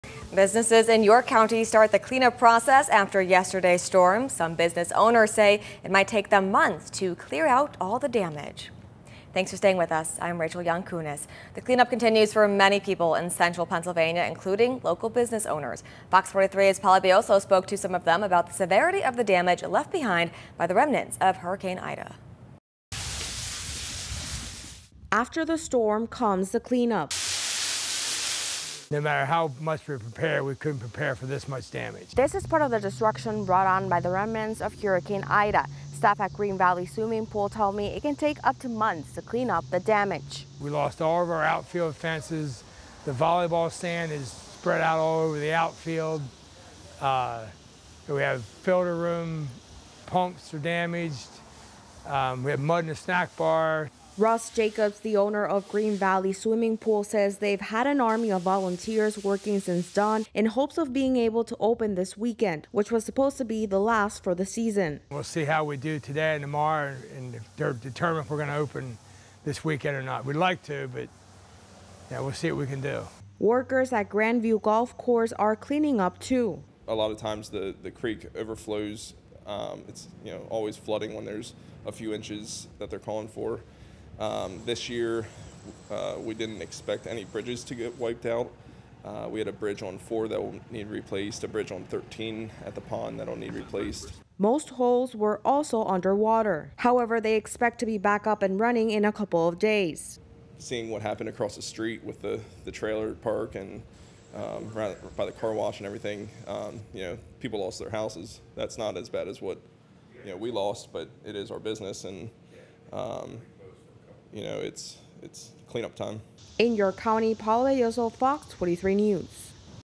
Businesses start the clean up process after Wednesday's storm
After the storm comes the cleanup.

One day after Tropical Depression Ida made its way through Central Pennsylvania, area businesses were assessing the damage and beginning repairs